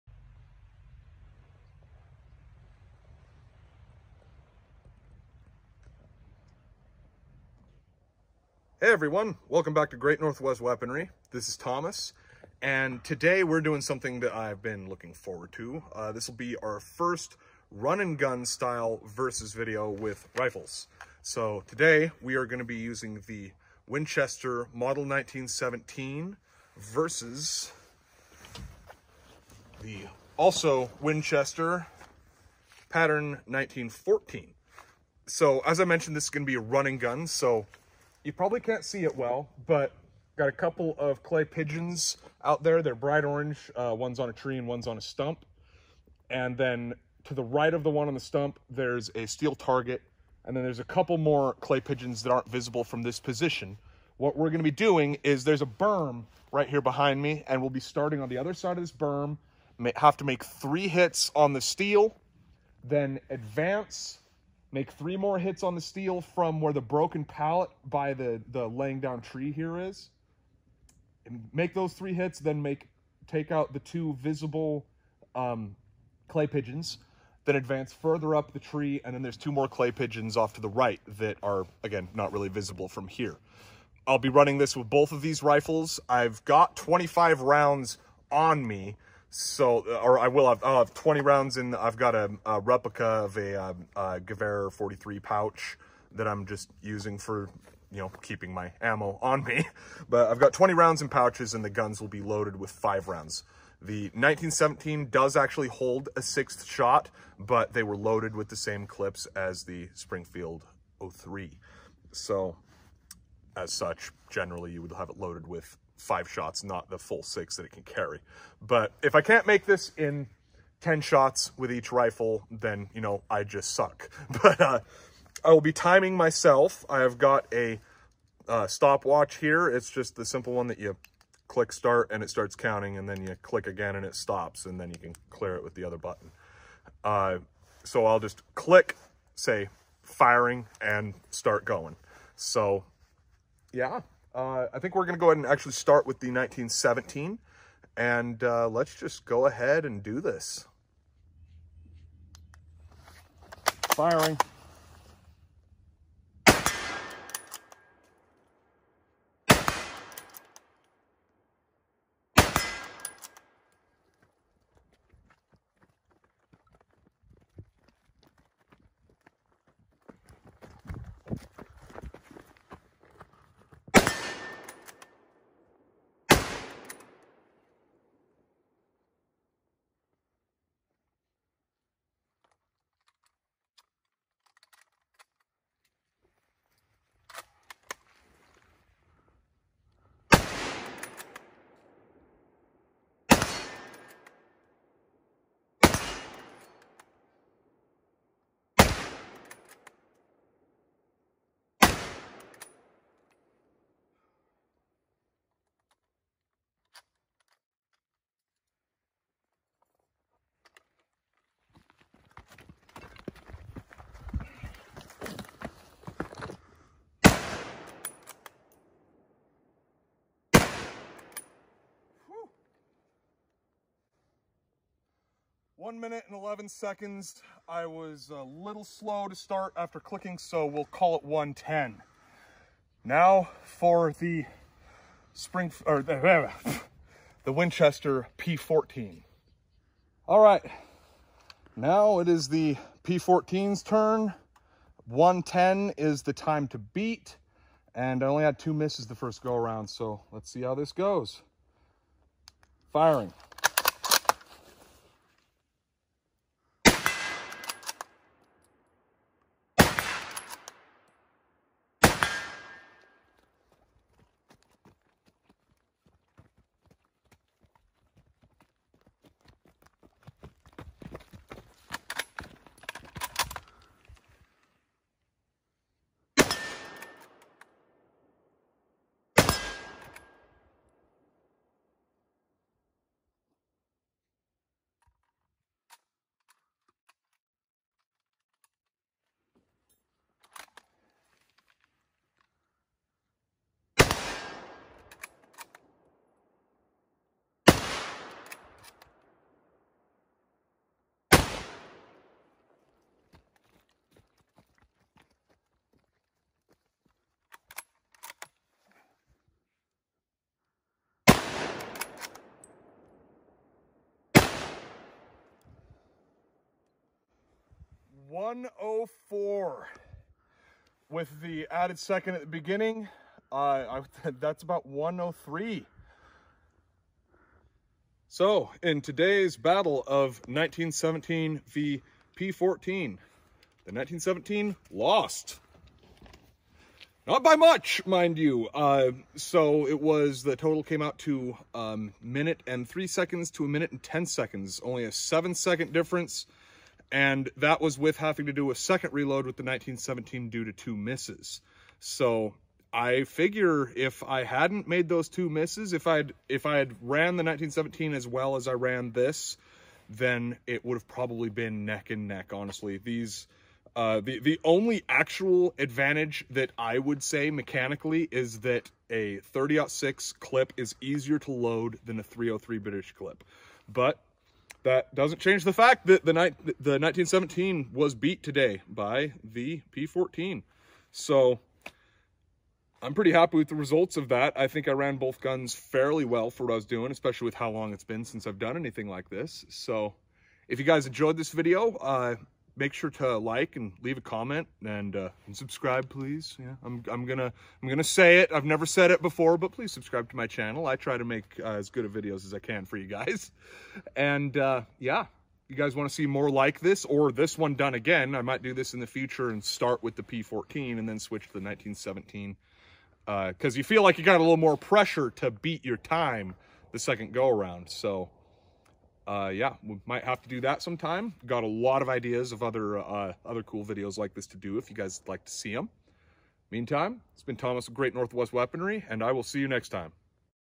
Winchester M1917 Vs. Winchester P14
Our first run and gun style versus video! Looking at a pair of WWI Winchesters, the Model 1917 and Pattern 1914!